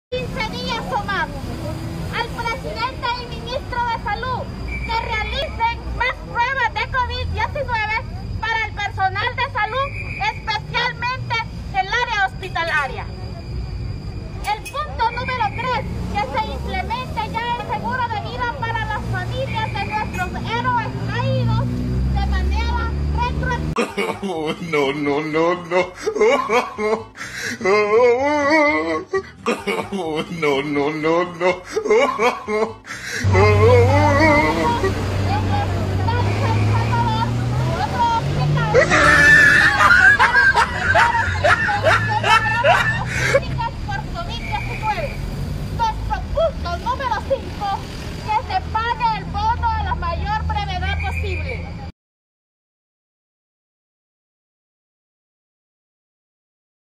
De metido en las cámara de canal 12. // CHUTEVISION
Cuando sales de chute en las cámaras ddel noticiero canall 12 en unaa protesta de médicos que exigían material de protección al gobierno Salvadoreño para no ser contagiados por Covid19